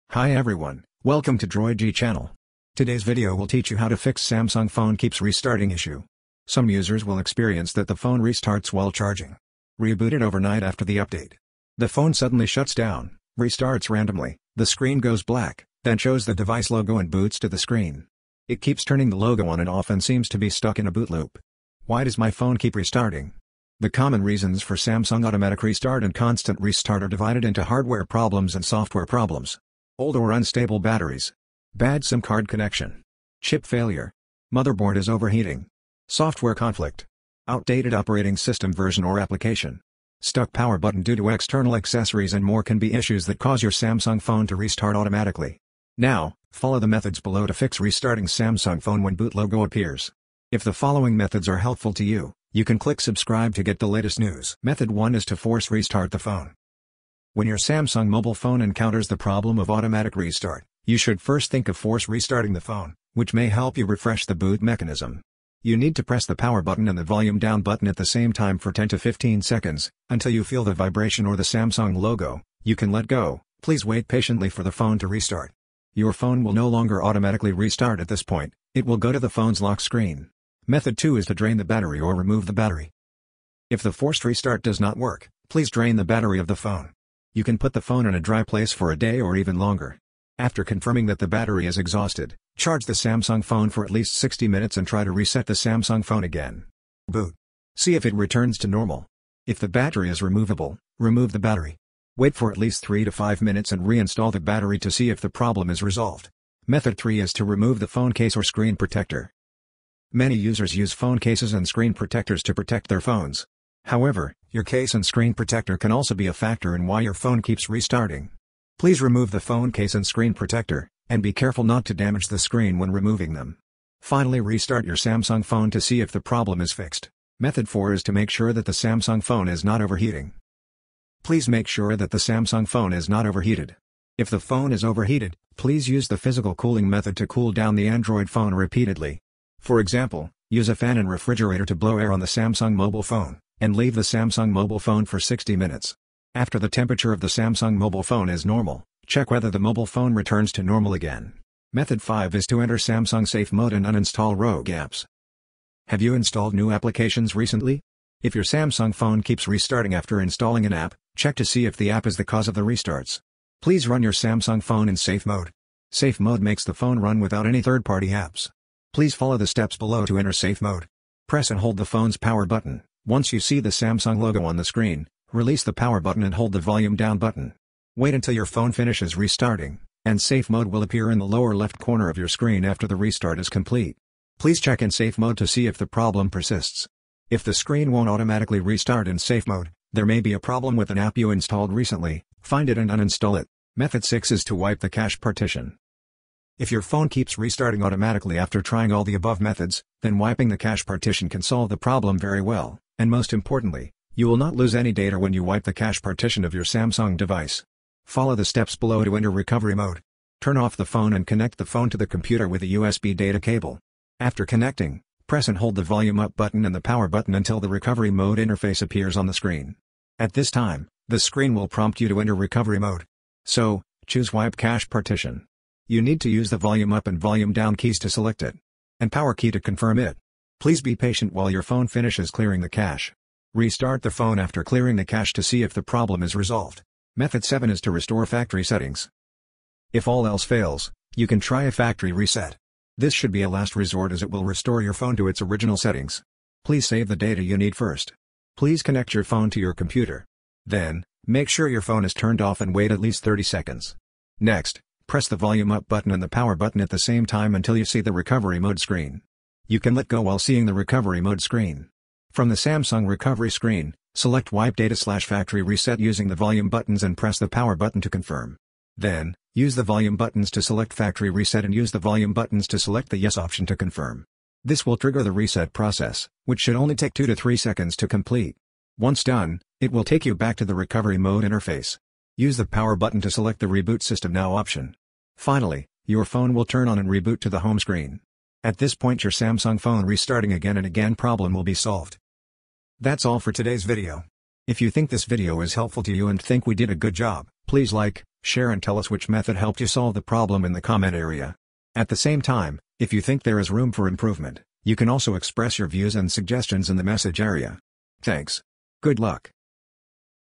Samsung Phone Keeps Restarting? Try This to Stop Phone Rebooting Itself Over & Over Randomly
Today's video will teach you how to fix Samsung phone keeps restarting.
Some users experience various Samsung phones that keep restarting randomly or repeatedly. The following situations may be the problems encountered by your mobile phone.

- The phone restarts while charging.
- Overnight reboot after update.
- The phone suddenly shuts down, restarts randomly, the screen goes black, then shows the device logo and boots to the screen.
- It keeps turning the logo on and off and seems to be stuck in a boot loop.

Why is your Android phone restarting on its own over and over again? This is a question we should think about. Only by understanding the reason can we better solve the problem. The common reasons for Samsung automatic restart and constant restart are divided into hardware problems and software problems. Please join me to understand the reasons why Samsung phones keep rebooting again and again. appears below.

- Old or unstable batteries.
- Bad SIM card connection.
- Chip failure.
- Motherboard is overheating.
- Software conflict.
- Outdated operating system versions or applications.
- The power button is stuck due to an external accessory.

After roughly understanding the reason, it is time to think about
How to fix an Android phone that keeps rebooting by itself? You can click on the timestamp below to see how to fix a Samsung phone that keeps rebooting at boot logo.

Chapters：

00:54 Force restart  Samsung phone
01:20 drain the battery or remove the battery
01:46 Clean screen
02:07 make sure that the Samsung phone is not overheating
02:31 Boot in safe mode
03:13 wipe cache partition
03:57 Restore factory settings